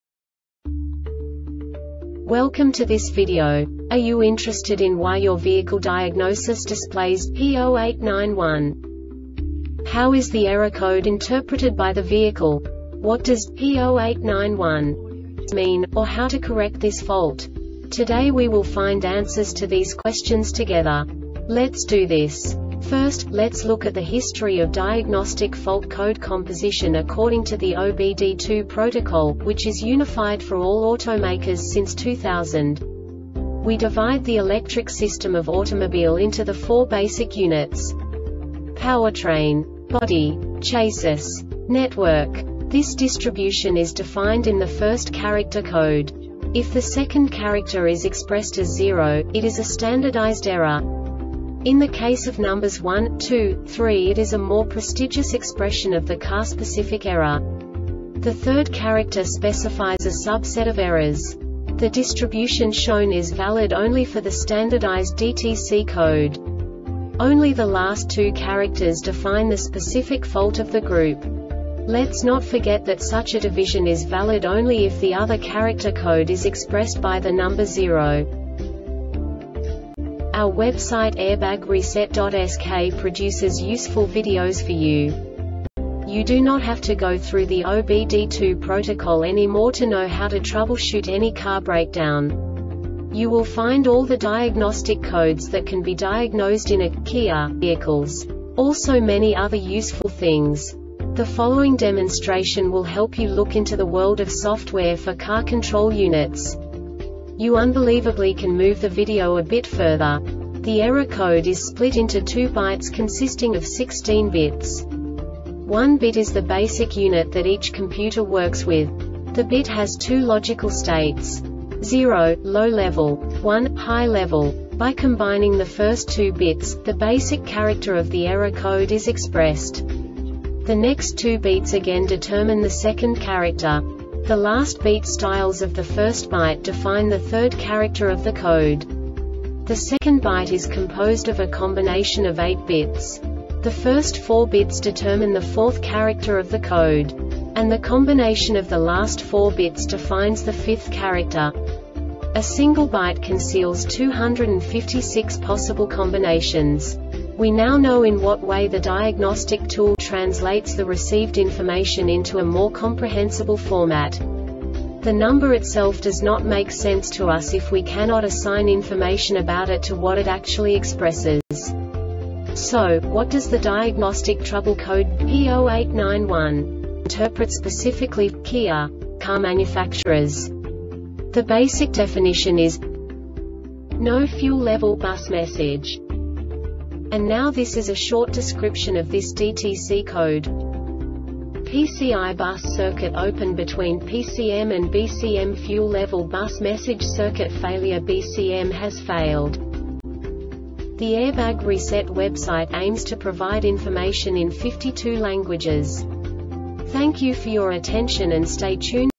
DTC KIA P0891 Short Explanation
Contents:
0:21 Basic DTC analysis according to OBD2 protocol standard.
1:48 Insight into programming
2:58 A diagnostically understandable description of the Diagnostic Trouble Code
3:05 Basic error definition

Make
Kia
Code
P0891
Definition
No Fuel Level Bus Message
Cause